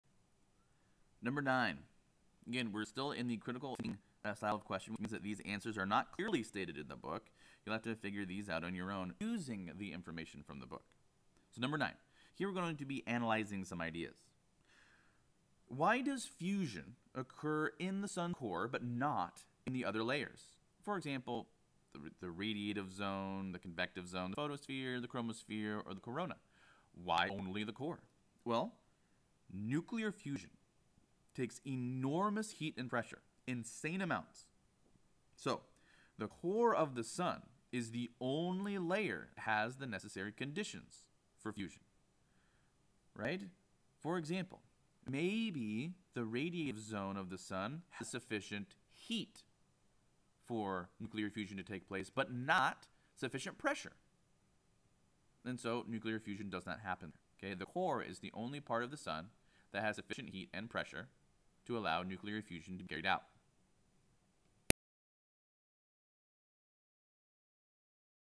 ES Ch 29-1 Section Review #9
9. Analyzing Ideas Why does fusion occur in the sun’s core but not in other layers?